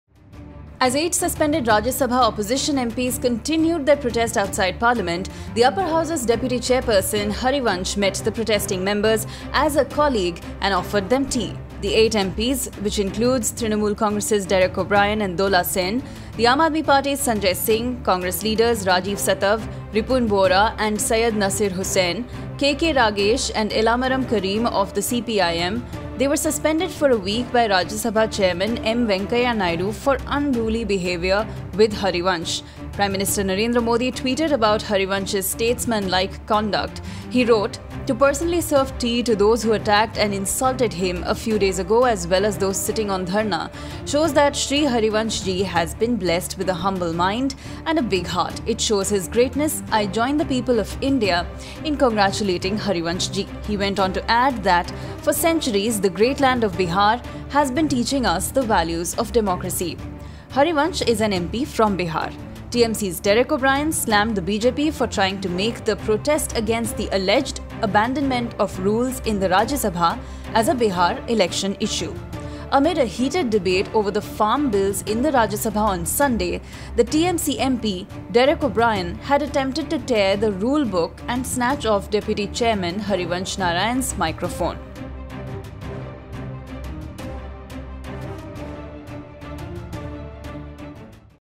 Derek O'Brien smells something fishy in tea offer by Harivansh | Oneindia News
As 8 suspended Rajya Sabha opposition MPs continued their protest outside Parliament, the upper house's deputy chairperson Harivansh met the protesting members as a colleague and offered them tea. The 8 MPs were suspended for a week by Rajya Sabha chairman M Venkaiah Naidu for “unruly behaviour” with Harivansh. However, not all MPs were pleased with the outreach.